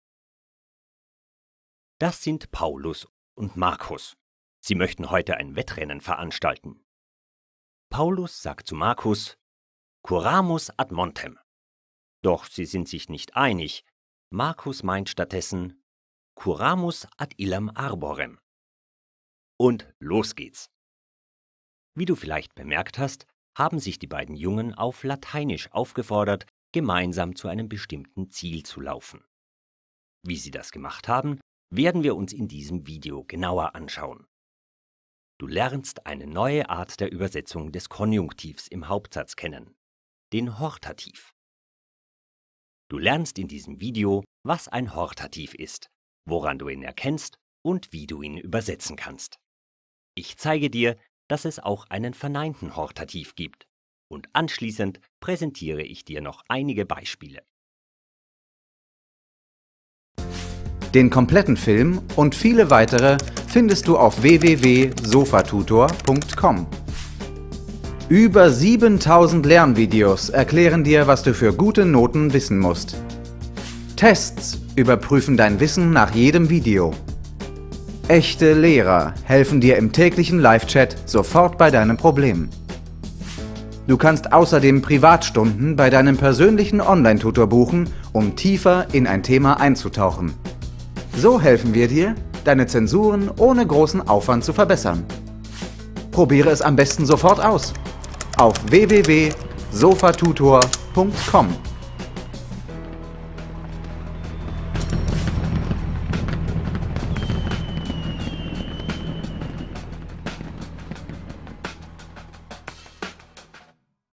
Den Hortativ bilden | Latein | Satzbau und Satzarten
Wenn du jemanden auf Latein zu etwas auffordern möchtest, benutzt du den Hortativ. Im Video lernst du, was der Hortativ ist, wie man ihn bildet und wie du ihn bestmöglich übersetzen solltest.

Dieses Video zum Thema Begehrsätze gehört zu: Latein | Satzbau und Satzarten | Begehrsätze